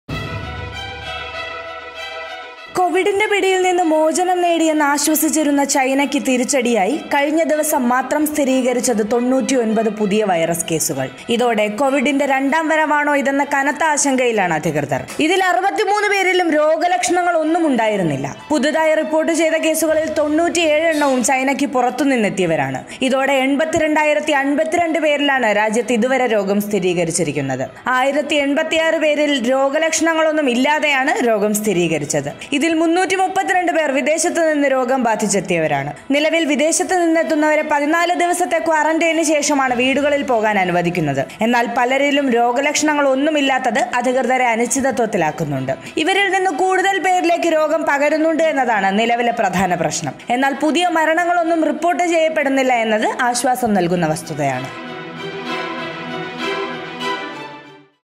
ഒറ്റ ദിവസം 99 കേസുകൾ; ചൈനയിലിത് കൊവിഡിന്റെ രണ്ടാം വരവോ
China reports 99 new COVID 19 cases